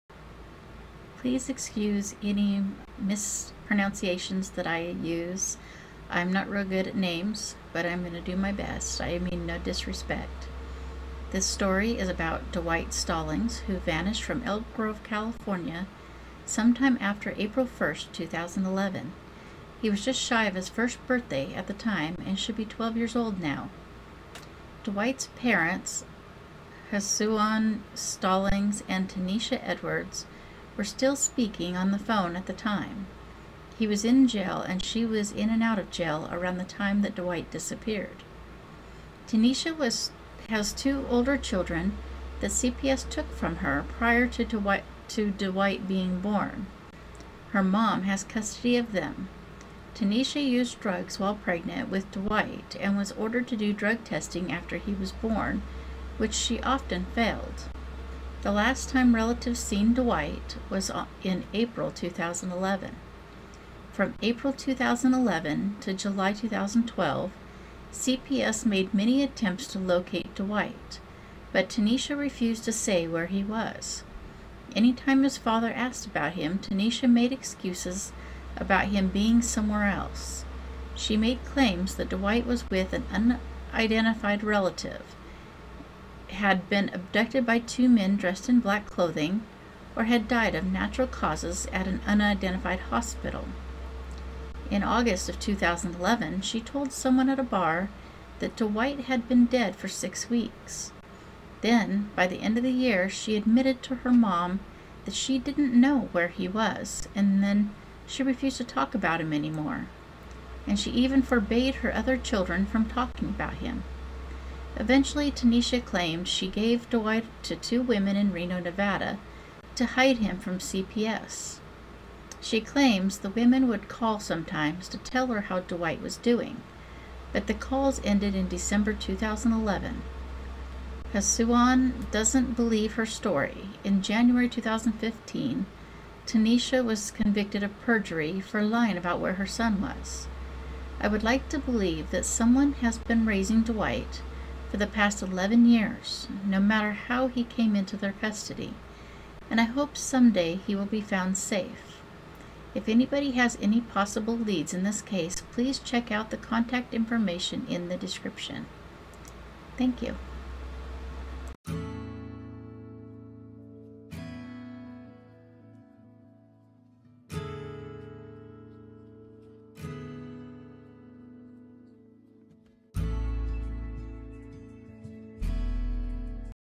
Dwight Stallings * Missing Since Possibly April 2011 * Elk Grove California
Endscreen music:
Bronsha Maurice